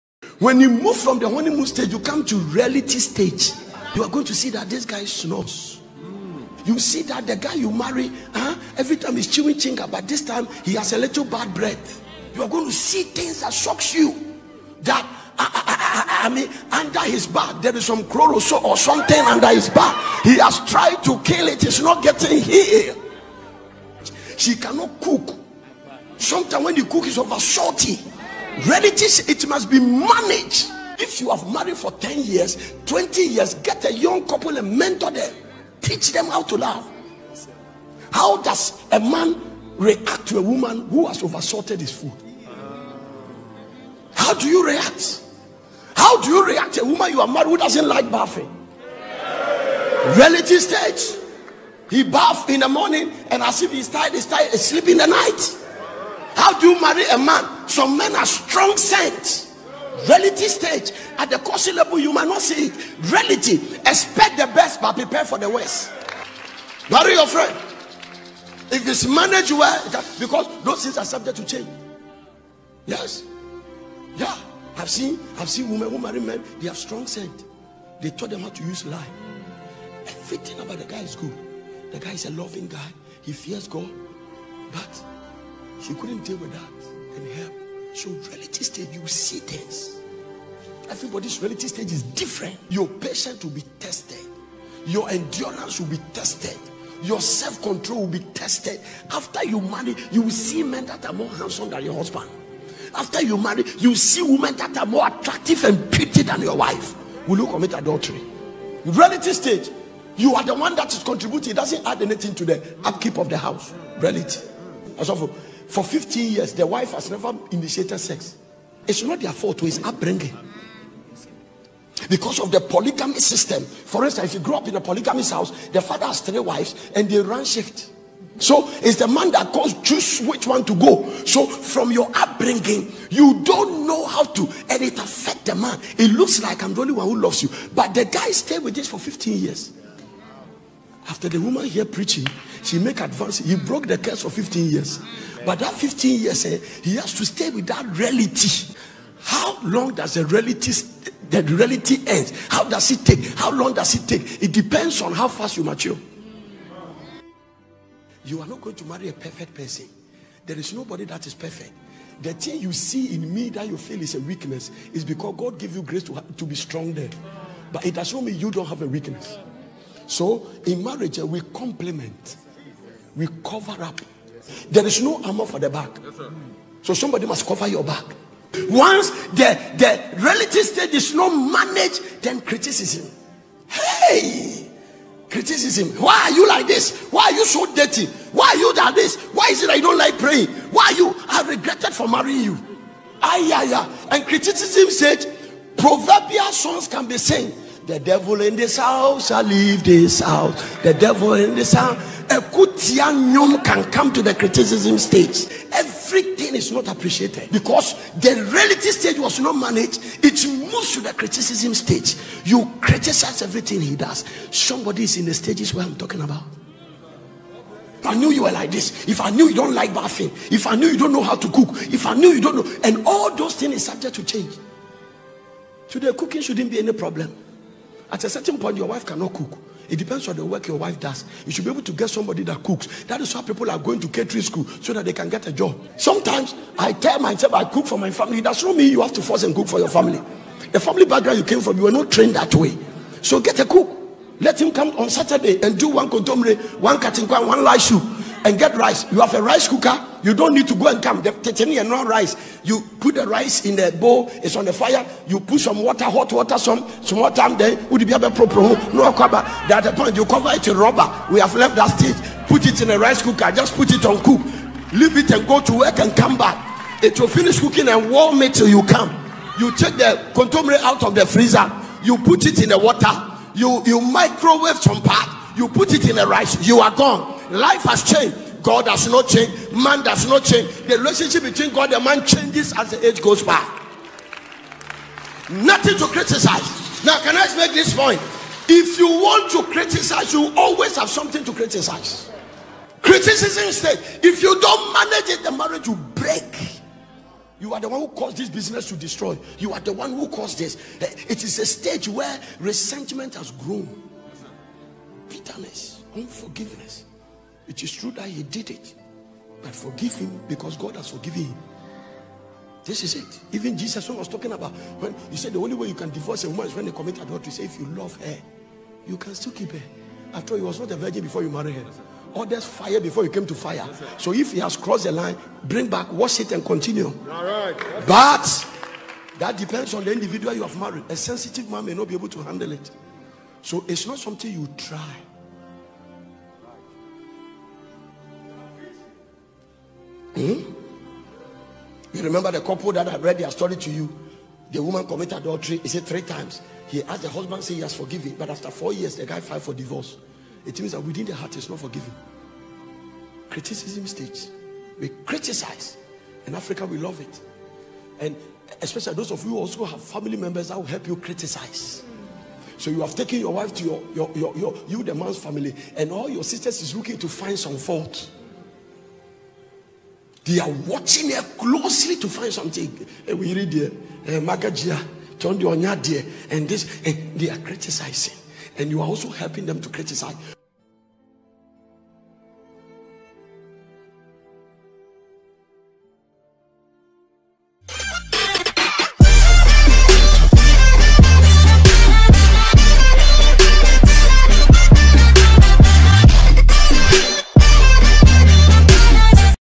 THINGS YOU WILL NEVER KNOW BEFORE YOU SAY I DO - Prophet Nanasei Opoku Sarkodie
Stages of marriage , see the practical realities of what marriage is all about .
How do deal with this realities when you come face to face with them . Prophet Nanasei Opoku Sarkodie
THINGS YOU WILL NEVER KNOW BEFORE YOU SAY I DO